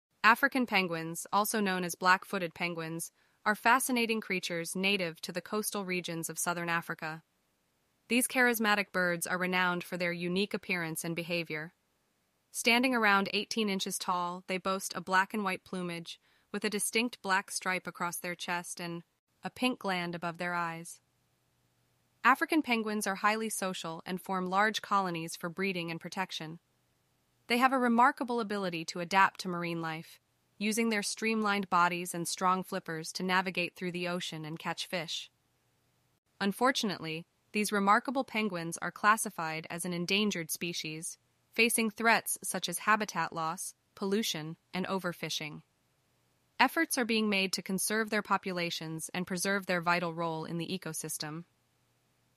A Look into the Lives of African Penguins 😮😮🐧
African penguins, also known as black-footed penguins, are charismatic creatures that capture the hearts of nature enthusiasts around the world. With their unique appearance, they stand out among other penguin species.

These flightless birds have a sleek black and white body with a distinct horseshoe-shaped black stripe across their chests. Native to the coastal areas of South Africa and Namibia, they thrive in rocky shorelines and islands. African penguins are highly social animals, forming tight-knit colonies where they engage in various activities like nesting, feeding, and preening.

Unfortunately, they face numerous threats, including habitat loss, pollution, and overfishing, making conservation efforts crucial to their survival.